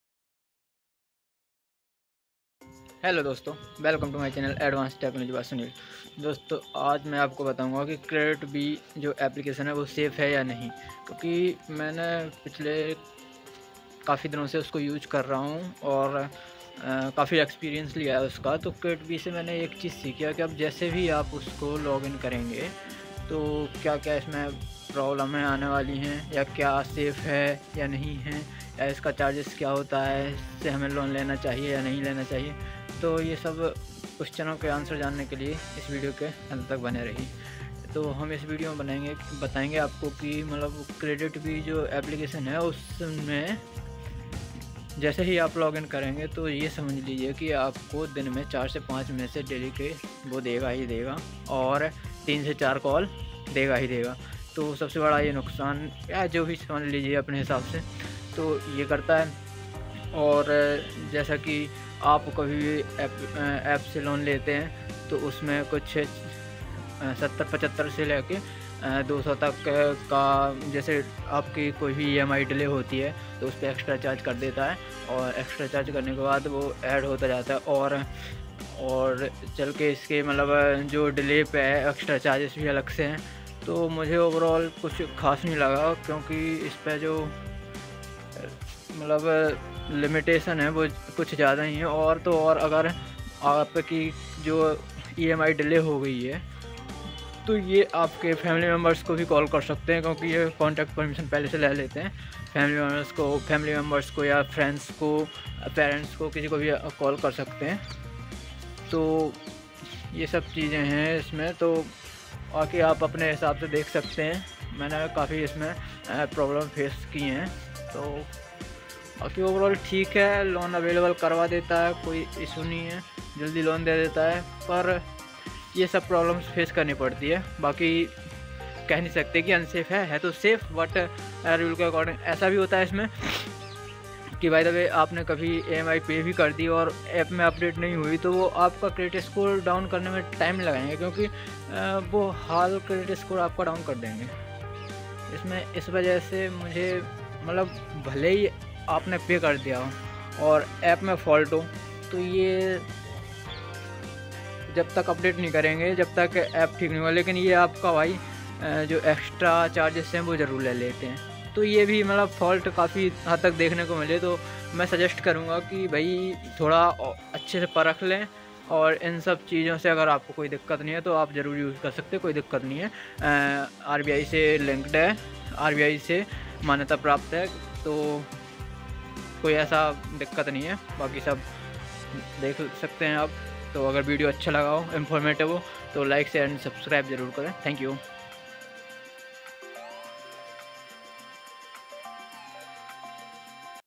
KreditBee loan app safe or not safe full review with my experience (New update)
Watch this complete video to know about kreditbee application this app safe or not safe know everything with my own experience in this app so watch completed thanks 🙏